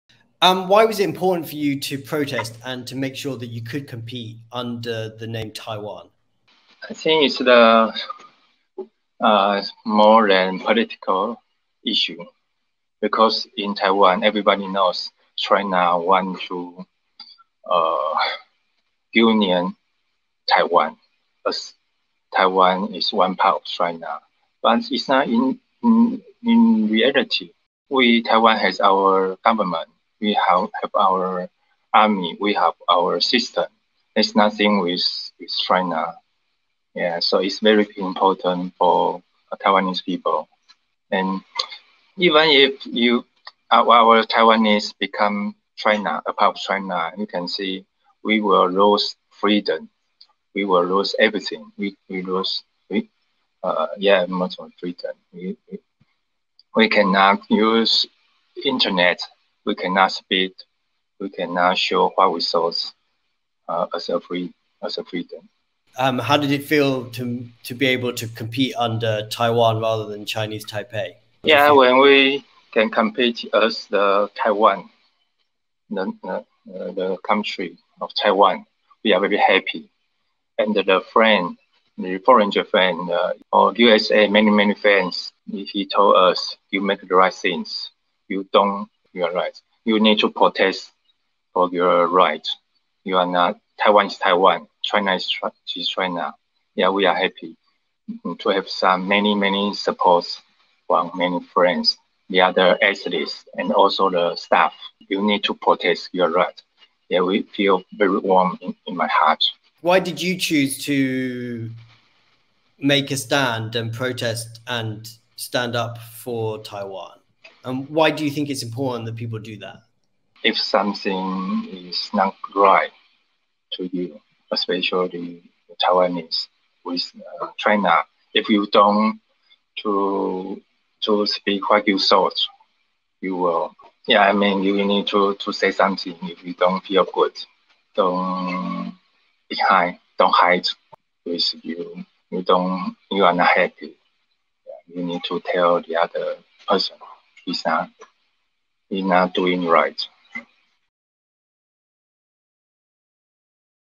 Taiwan Protest at Freediving Cup | TaiwanPlus News
📹Reporters: Rik Glauert/Kama Hsu

Taiwanese athletes competing at the Caribbean Cup in Honduras last week protested competing under the name Chinese Taipei. The participants of the freediving world series threatened to register “did not starts” at the international competition. Reporter Rik Glauert spoke to Taiwanese freediver Huang Ming-chun, who came second in his category, about the protest. He began by asking him why competing under the name Taiwan was worth risking leaving the competition for: